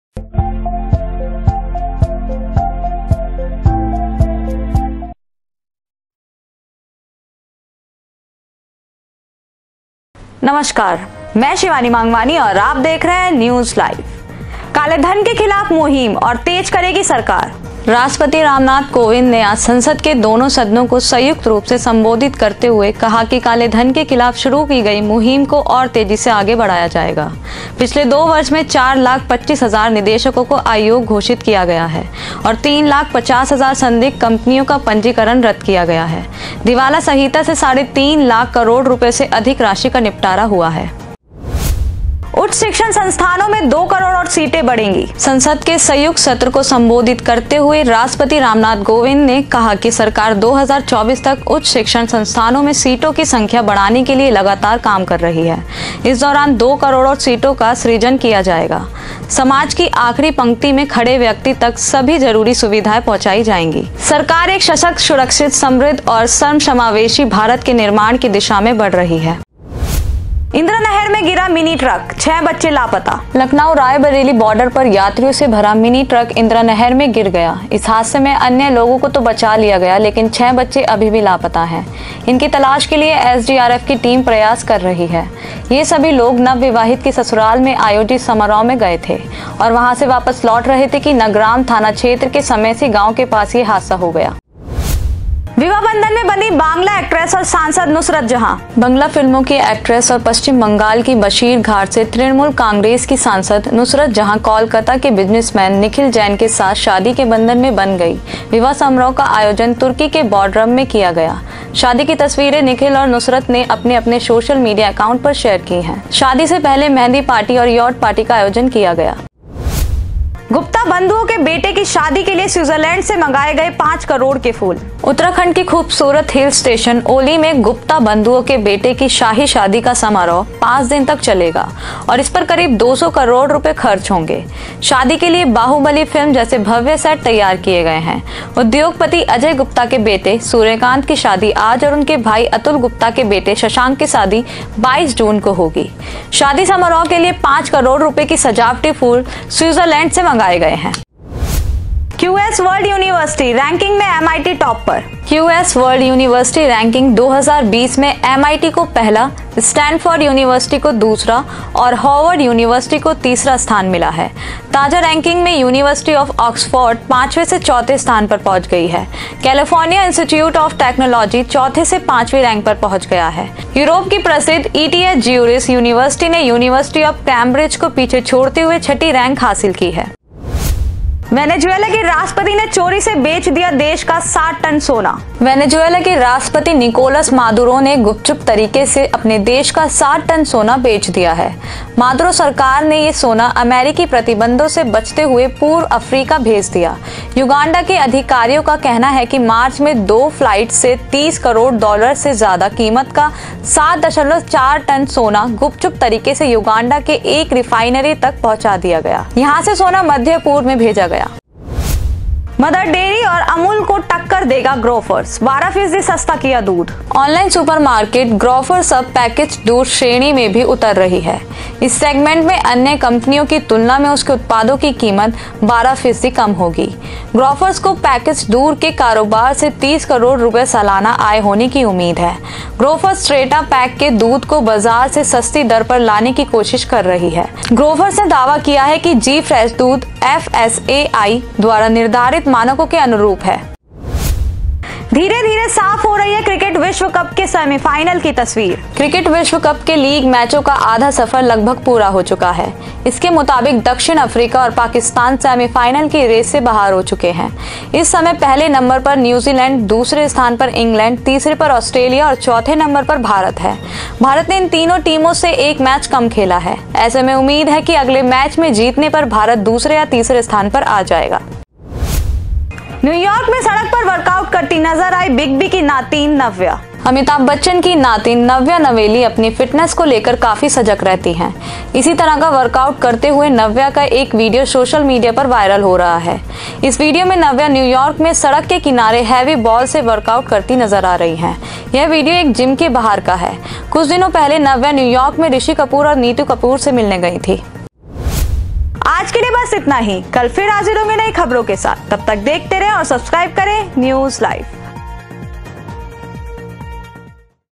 News Live || 20-June-2019 || IIMT Media || काले धन के खिलाफ मुहिम और तेज करेगी सरकार
काले धन के खिलाफ मुहिम और तेज करेगी सरकार
राष्ट्रपति रामनाथ कोविंद ने आज संसद के दोनों सदनों को संयुक्त रूप से संबोधित करते हुए कहा कि काले धन के खिलाफ शुरू की गई मुहिम को और तेजी से आगे बढ़ाया जाएगा। पिछले दो वर्ष में 4 लाख 25 हजार निदेशकों को अयोग्य घोषित किया गया है और 3 लाख 50 हजार संदिग्ध कंपनियों का पंजीकरण रद्द किया गया है। दिवाला संहिता से साढ़े 3 लाख करोड़ रुपए से अधिक राशि का निपटारा हुआ है।

उच्च शिक्षण संस्थानों में दो करोड़ और सीटें बढ़ेंगी
संसद के संयुक्त सत्र को संबोधित करते हुए राष्ट्रपति रामनाथ कोविंद ने कहा कि सरकार 2024 तक उच्च शिक्षण संस्थानों में सीटों की संख्या बढ़ाने के लिए लगातार काम कर रही है। इस दौरान दो करोड़ और सीटों का सृजन किया जाएगा। समाज की आखिरी पंक्ति में खड़े व्यक्ति तक सभी जरुरी सुविधाएं पहुंचाई जाएंगी। सरकार एक सशक्त, सुरक्षित, समृद्ध और सर्वसमावेशी भारत के निर्माण की दिशा में बढ़ रही है।

इंदिरा नहर में गिरा मिनी ट्रक, छह बच्चे लापता
लखनऊ-रायबरेली बॉर्डर पर यात्रियों से भरा मिनी ट्रक इंदिरा नहर में गिर गया। इस हादसे में अन्य लोगों को तो बचा लिया गया लेकिन छह बच्चे अभी भी लापता हैं। इनकी तलाश के लिए एसडीआरएफ की टीम प्रयास कर रही है। ये सभी लोग नवविवाहिता की ससुराल में आयोजित समारोह में गए थे और वहां से वापस लौट रहे थे कि नगराम थाना क्षेत्र के समेसी गॉव के पास ये हादसा हो गया।

विवाह बंधन में बंधीं बांग्ला एक्ट्रेस और सांसद नुसरत जहां
बांग्ला फिल्मों की एक्ट्रेस और पश्चिम बंगाल के बशीरघाट से तृणमूल कांग्रेस की सांसद नुसरत जहां कोलकाता के बिजनसमैन निखिल जैन के साथ शादी के बंधन में बंध गईं। विवाह समारोह का आयोजन तुर्की के बोडरम में किया गया। शादी की तस्वीरें निखिल और नुसरत ने अपने-अपने सोशल मीडिया एकाउंट पर शेयर की हैं। शादी से पहले मेंहदी पार्टी और यॉट पार्टी का आयोजन किया गया।

गुप्ता बंधुओं के बेटों की शादी के लिए स्विट्जरलैंड से मंगाए 5 करोड़ के फूल
उत्तराखंड के खूबसूरत हिल स्टेशन औली में गुप्ता बंधुओं के बेटों की शाही शादी का समारोह 5 दिनों तक चलेगा और इस पर करीब 200 करोड़ रुपये खर्च होंगे। शादी के लिए बाहुबली फिल्म जैसे भव्य सेट तैयार किए गए हैं। उद्योगपति अजय गुप्ता के बेटे सूर्यकांत की शादी आज और उनके भाई अतुल गुप्ता के बेटे शशांक की शादी 22 जून को होगी। शादी समारोह के लिए 5 करोड़ रुपये के सजावटी फूल स्विटजरलैंड से मंगाए गए हैं।

क्यूएस वर्ल्ड यूनिवर्सिटी रैंकिंग में एमआईटी टाप पर
क्यूएस वर्ल्ड यूनिवर्सिटी रैंकिंग 2020 में एमआईटी को पहला, स्टेनफोर्ड यूनिवर्सिटी को दूसरा और हार्वर्ड यूनिवर्सिटी को तीसरा स्थान मिला है। ताजा रैंकिंग में यूनिवर्सिटी ऑफ ऑक्सफोर्ड पांचवें से चौथे स्थान पर पहुंच गई है। कैलिफोर्निया इंस्टीट्यूट ऑफ टेक्नोलॉजी चौथी से पांचवीं रैंक पर पहुंच गया है। यूरोप की प्रसिद्ध ईटीएच ज्यूरिच यूनिवर्सिटी ने यूनिवर्सिटी ऑफ कैंब्रिज को पीछे छोड़ते हुए छठी रैंक हासिल की है।

वेनेजुएला के राष्ट्रपति ने चोरी से बेच दिया देश का 7 टन सोना
वेनेजुएला के राष्ट्रपति निकोलस मादुरो ने गुपचुप तरीके से अपने देश का सात टन सोना बेच दिया है। मादुरो सरकार ने ये सोना अमेरिकी प्रतिबंधों से बचते हुए पूर्व अफ्रीका भेज दिया। युगांडा के अधिकारियों का कहना है कि मार्च में दो फ्लाइट्स से 30 करोड़ डॉलर से ज्यादा कीमत का 7.4 टन सोना गुपचुप तरीक से युगांडा की एक रिफाइनरी तक पहुंचा दिया गया। यहां से सोना मध्य-पूर्व में भेजा गया।

मदर डेयरी और अमूल को टक्कर देगा ग्रोफर्स, 12 फीसदी सस्ता किया दूध
ऑनलाइन सुपरमार्केट ग्रोफर्स अब पैकेज्ड दूध श्रेणी में भी उतर रही है। इस सेगमेंट में अन्य कंपनियों की तुलना में उसके उत्पादों की कीमत 12 फीसदी कम होगी। ग्रोफर्स को पैकेज्ड दूध के कारोबार से 30 करोड़ रुपये सालाना आय होने की उम्मीद है। ग्रोफर्स टेट्रा पैक के दूध को बाजार से सस्ती दर पर लाने की कोशिश कर रही है। ग्रोफर्स ने दावा किया कि ‘जी-फ्रेश’ दूध एफएसएसएआई द्वारा निर्धारित मानकों के अनुरूप है।

धीरे-धीरे साफ हो रही है क्रिकेट विश्व कप के सेमीफाइल की तस्वीर
क्रिकेट विश्व कप के लीग मैचों का आधा सफऱ लगभग पूरा हो चुका है। इसके मुताबिक दक्षिण अफ्रीका और पाकिस्तान सेमीफाइनल की रेस से बाहर हो चुके हैं। इस समय पहले नंबर पर न्यूजीलैंड, दूसरे स्थान पर इंग्लैंड, तीसरे पर ऑस्ट्रेलिया और चौथे नंबर पर भारत है। भारत ने इन तीनों टीमों से एक मैच कम खेला है। ऐसे में उम्मीद है कि अगले मैच में जीतने पर भारत दूसरे या तीसरे स्थान पर आ जाएगा।

न्यूयॉर्क में सड़क पर वर्कआउट करती नजर आईं बिग बी की नातिन नव्या
अमिताभ बच्चन की नातिन नव्या नवेली अपनी फिटनेस को लेकर काफी सजग रहती हैं। इसी तरह का वर्कआउट करते हुए नव्या का एक वीडियो सोशल मीडिया पर वायरल हो रहा है। इस वीडियो में नव्या न्यूयॉर्क में सड़क के किनारे हैवी बॉल से वर्कआउट करती नजर आ रही हैं। यह वीडियो एक जिम के बाहर का है। कुछ दिनों पहले नव्या न्यूयॉर्क में ऋषि कपूर और नीतू कपूर से मिलने गई थीं।